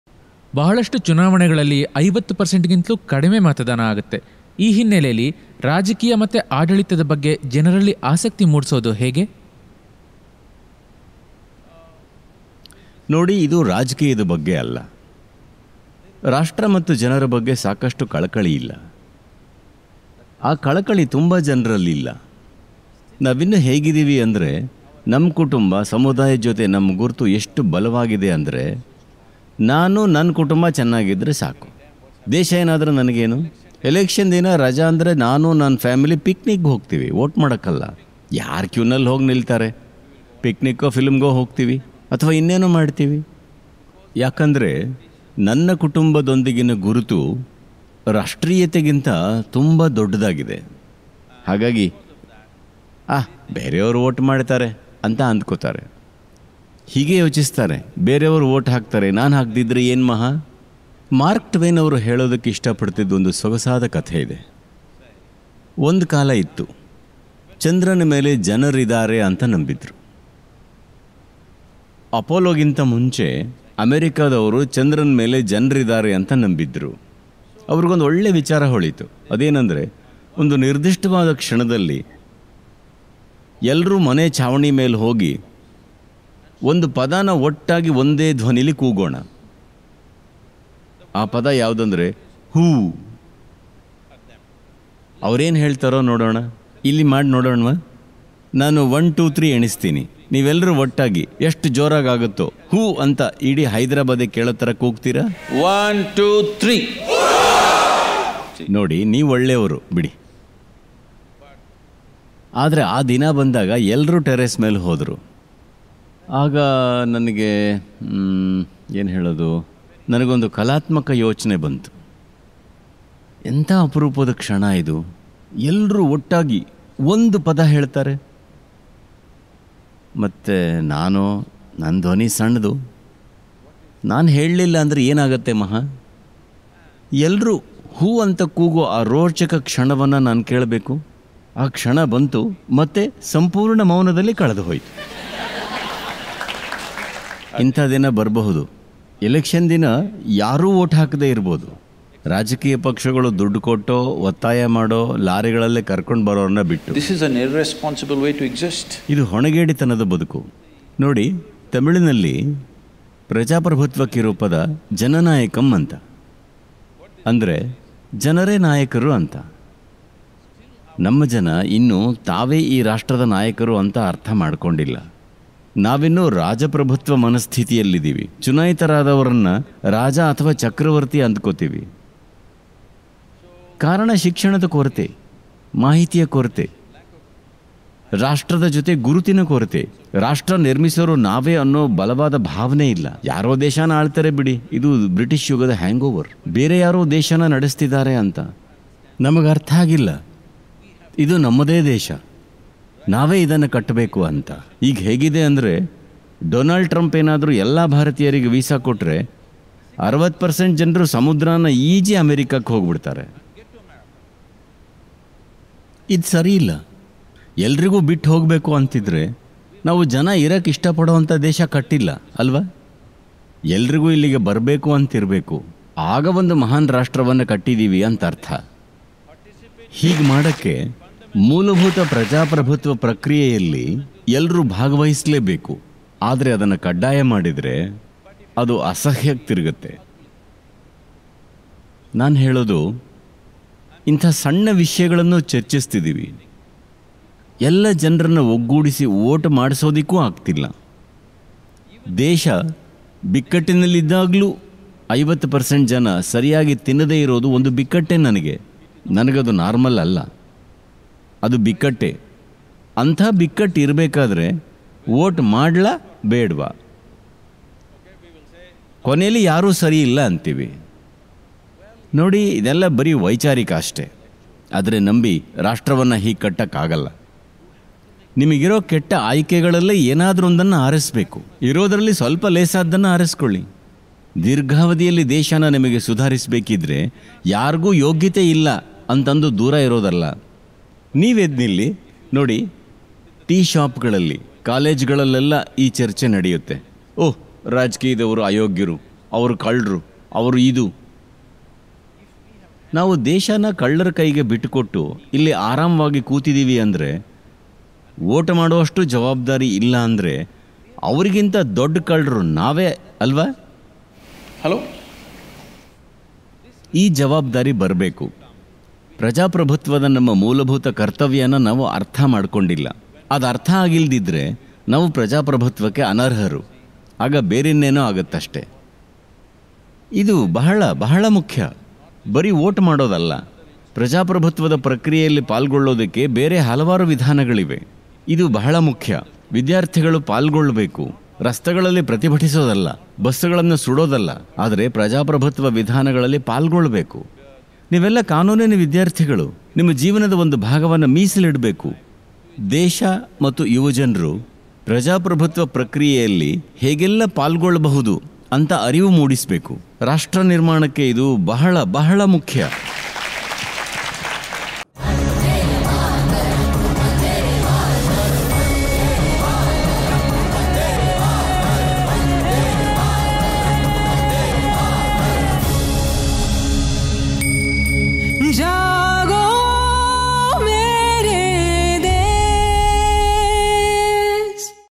ಯಾವ ಪಕ್ಷಾನೂ ಸರಿಯಿಲ್ಲ, ವೋಟ್ ಯಾಕೆ ಮಾಡ್ಲಿ? | ಸದ್ಗುರು
ಚುನಾವಣೆಗಳಲ್ಲಿ 50% ಗಿಂತ ಕಡಿಮೆ ಜನರು ಮತ ಚಲಾಯಿಸುತ್ತಾರೆ. ಇದನ್ನು ಸುಧಾರಿಸುವುದು ಹೇಗೆ ಎಂಬ ಬಗ್ಗೆ ಸದ್ಗುರು ಮಾತನಾಡುತ್ತಾ ಎಲ್ಲರಿಗೂ ತಮ್ಮ ಈ ಹಕ್ಕನ್ನು ಚಲಾಯಿಸಲು ಕರೆನೀಡುತ್ತಾರೆ.